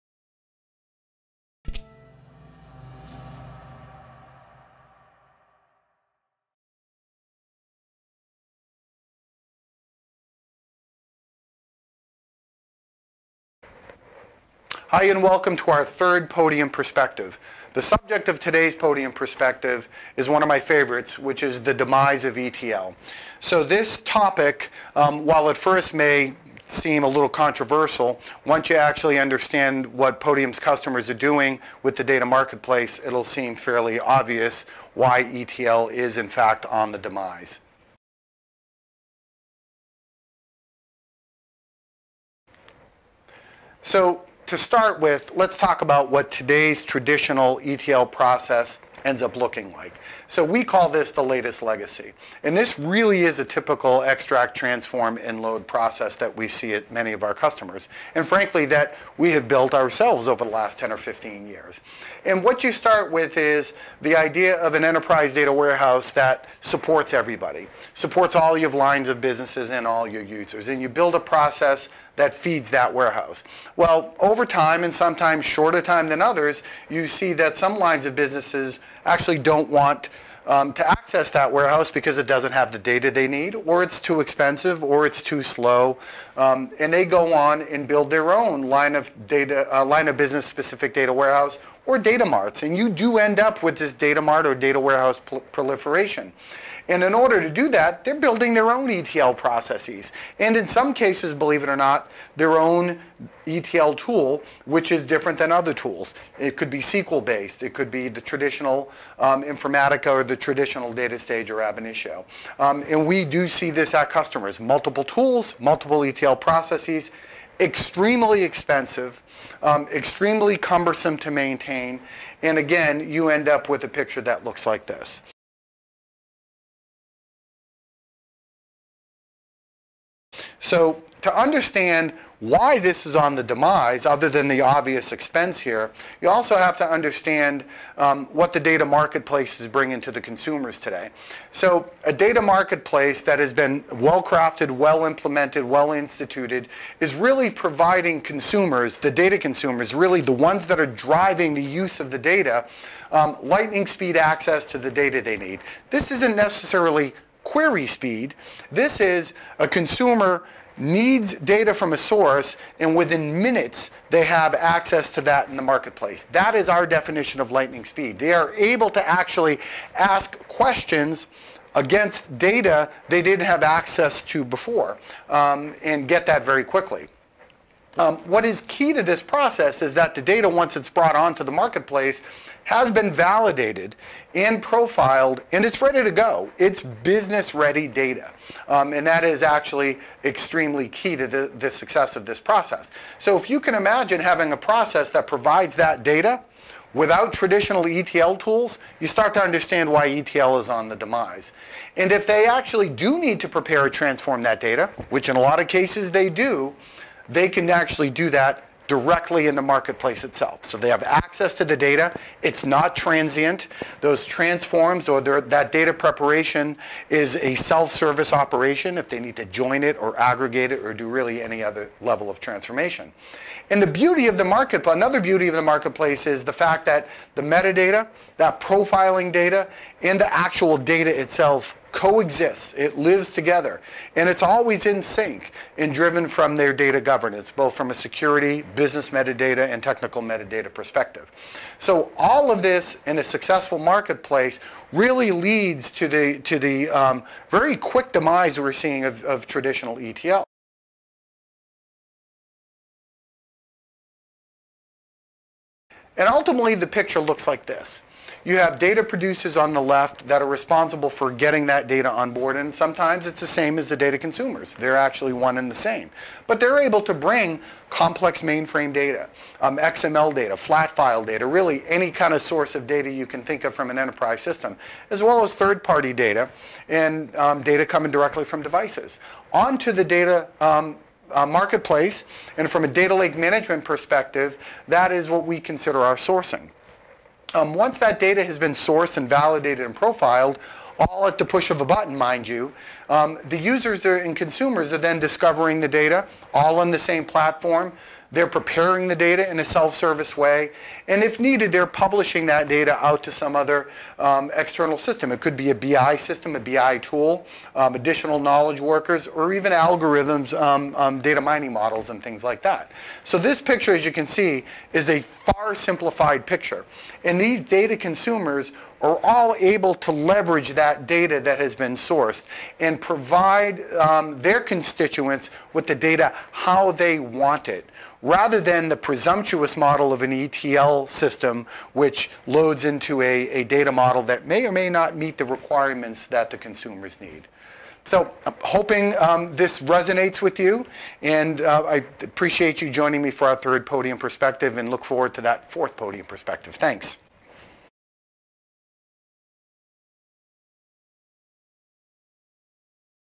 Podium Perspective: The Demise of ETL
Extraction/Transformation/Load tools were the de facto data preparation in the past, but in today's era of big data, self-service data prep and widespread analytics, they no longer meet the need, Podium CTO Bob Vecchione explains how using a data marketplace to prepare and deliver data lowers costs, simplifies the stack, and accelerates time to answer.